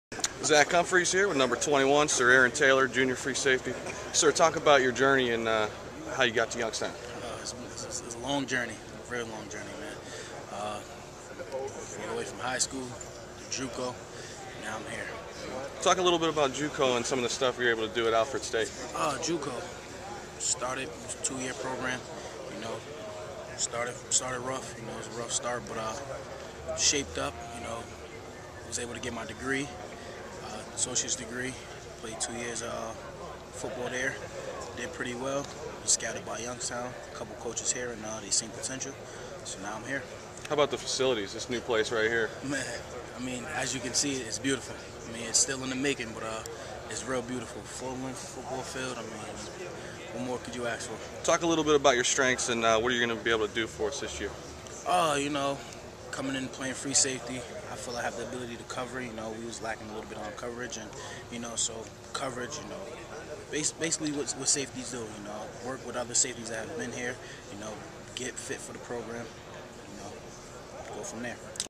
Sir Aaron Taylor | YSU Football | Aug. 6, 2011
Youngstown State junior free safety Sir Aaron Taylor talks with YSUsports.com on the Penguins' media day at the WATTS in Youngstown, Ohio.